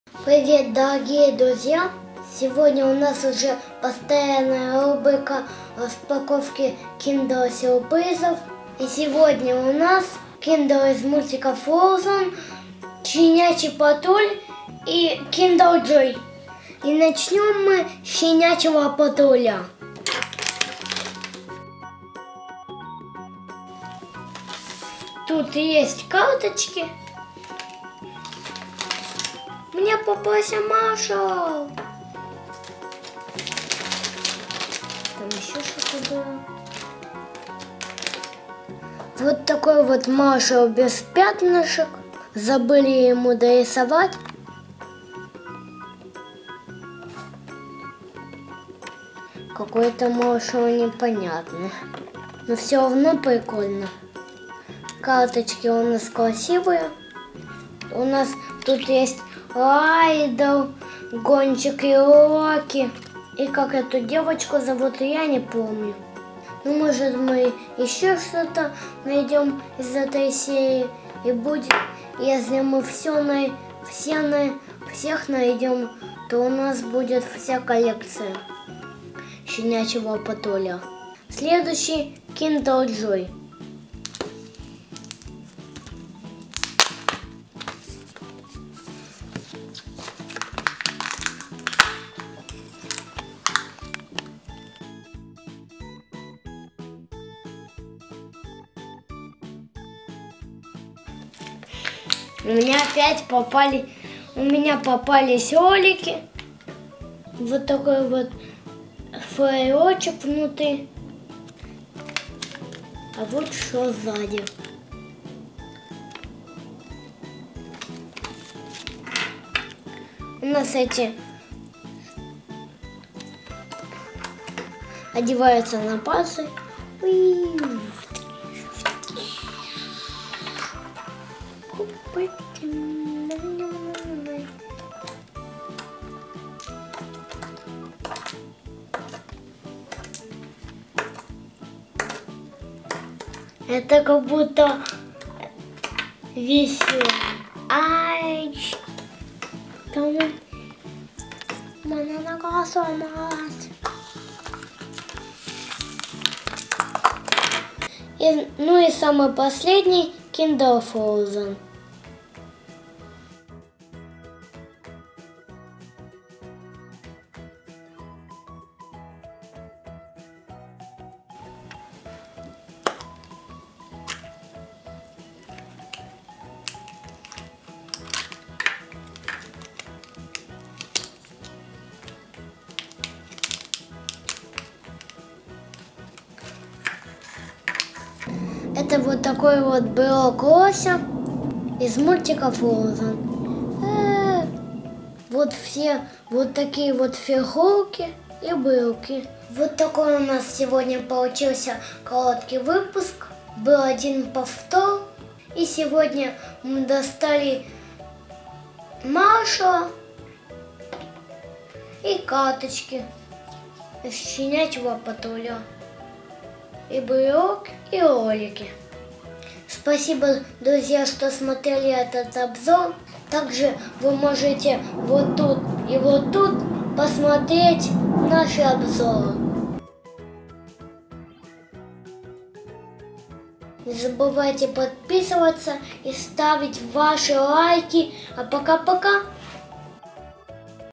SURPRISE EGGS #10. Kinder Сюрприз. Распаковка 3 сюрпризов.
Что же там внутри? Смотри вместе с нами и узнаешь.

Спасибо за помощь на развитие канала:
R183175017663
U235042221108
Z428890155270